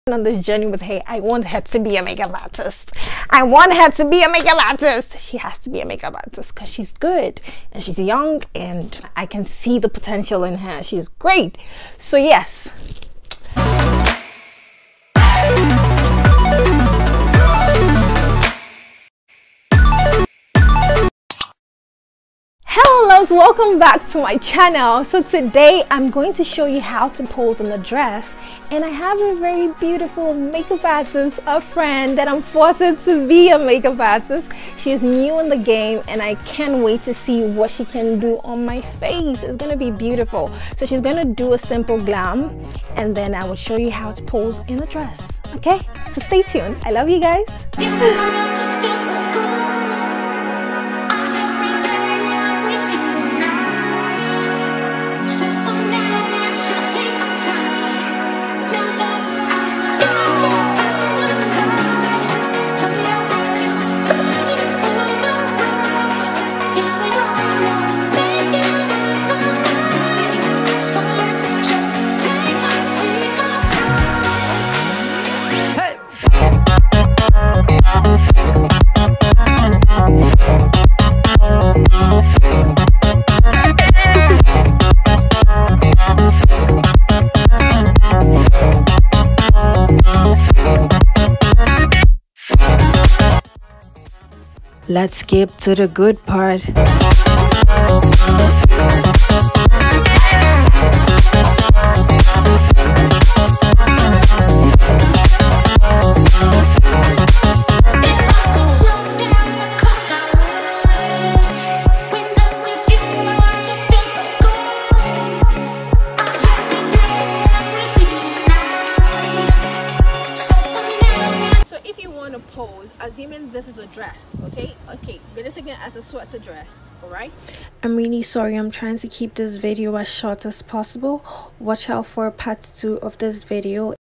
Part 1 of How to pose in a dress & Aida’s first time of doing makeup on me💃🏼💪🏽💯💄
Met this young girl, Aida not long ago and she did her makeup to perfection , so I decided to push her to do makeup on me because I believe she will make a great makeup artist selfdevelopment . To my surprise I am the first person she has ever done makeup on.

Also, in the part 2 of this video I am going to show you how to pose in a sweater which I pictured as a sweat dress

My Podcast